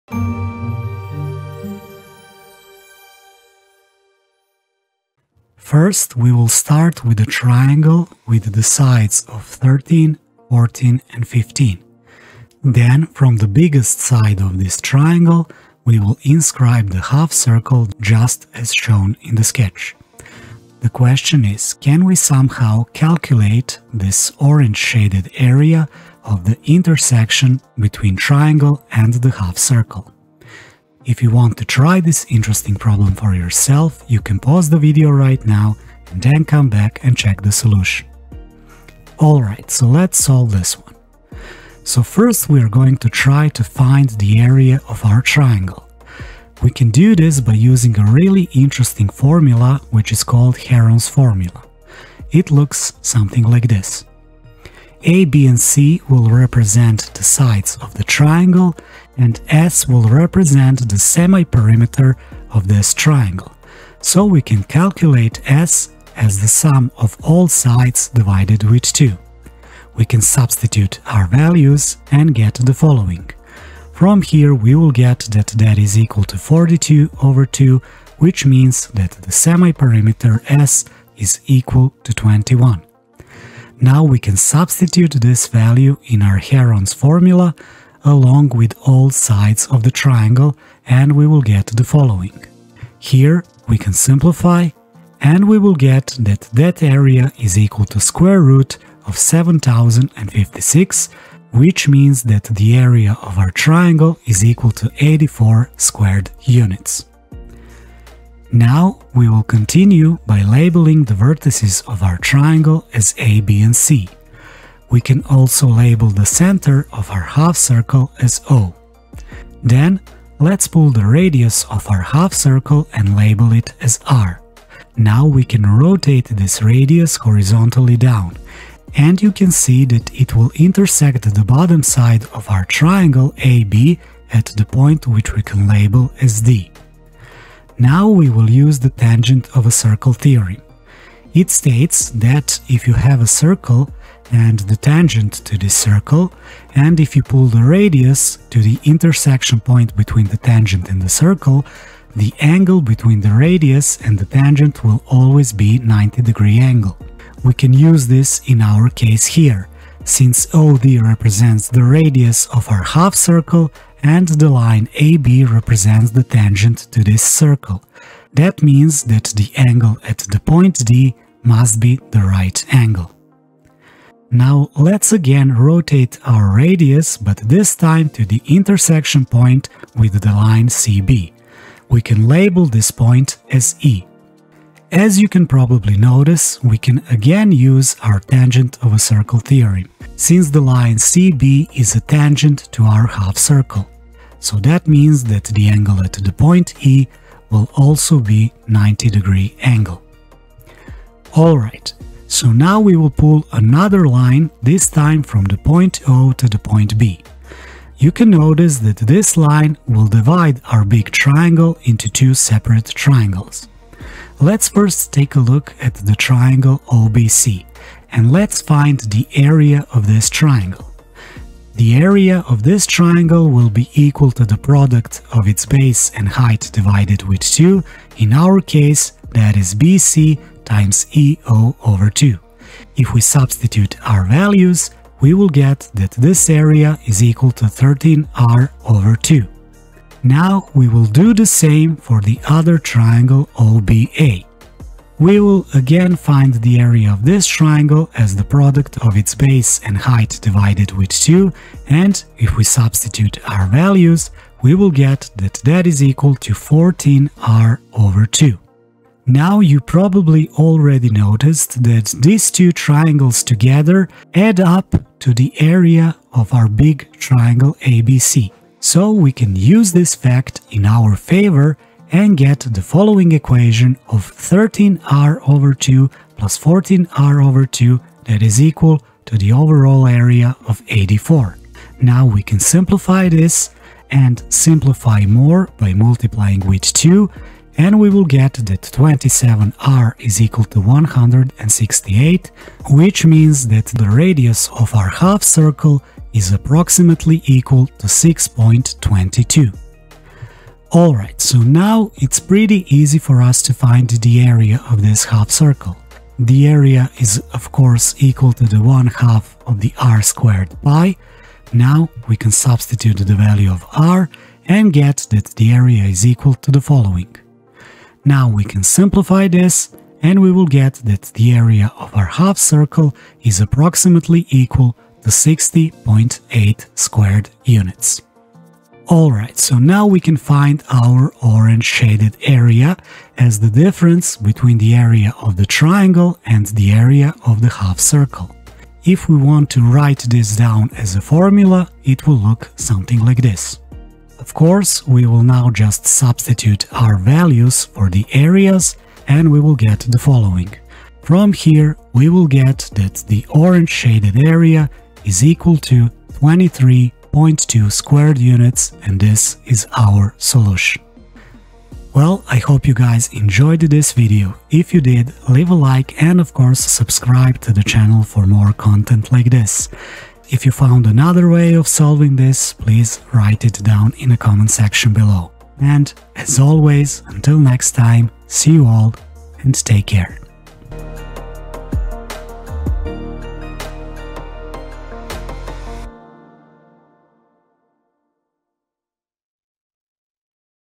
Can You Solve This Math Puzzle? | Orange Shaded Area in a Scalene Triangle
Welcome to another exciting geometry challenge! In this video, we explore a fascinating problem involving a scalene triangle and a half-circle, which together form an orange shaded area. Can you figure out the solution?

You'll be tasked with calculating the area of the orange region inside a triangle with sides measuring 13, 14, and 15. While it may look simple at first glance, this math puzzle will test your geometry skills and logical thinking.

Ready to take on the challenge? Watch the video and see if you can solve it before the solution is revealed!

Share your solution or questions in the comments below.